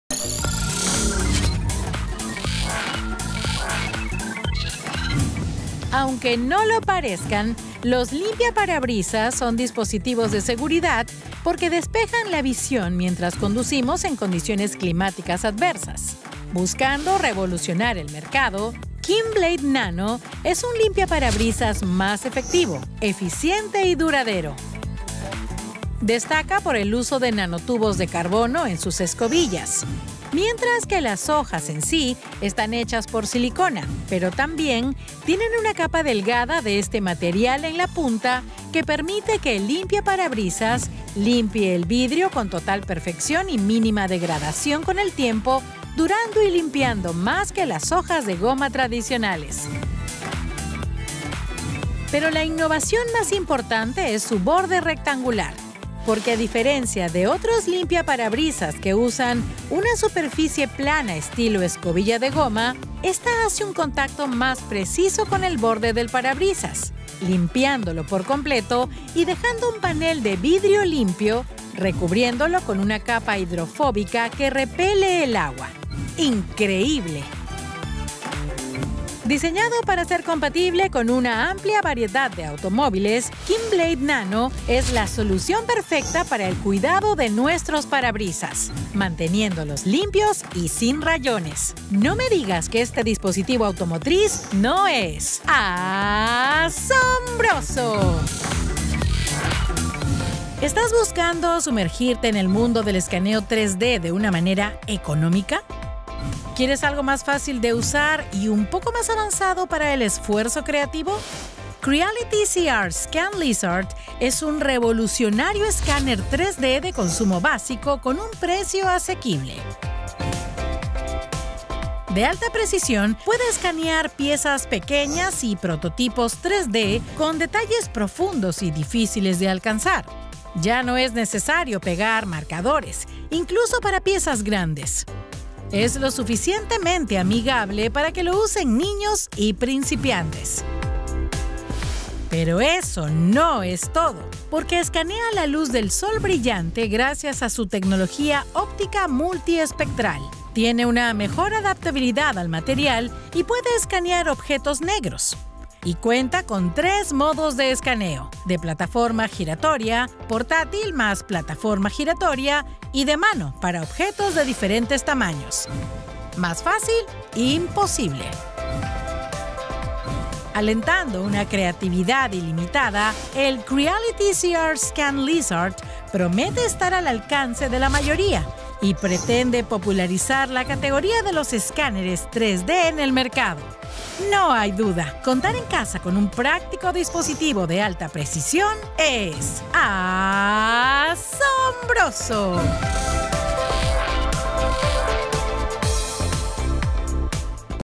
Asombroso!: Un scanner 3D fácil de usar
Todos conocemos las impresiones 3D, pero ¿sabías que también hay scanners 3D? CR-Scan Lizard 3D Scanner nos ofrece esta posibilidad con su scanner de alta precisión con el que podrás modelar lo que desees rápidamente en 3D para tener un diseño listo a imprimir.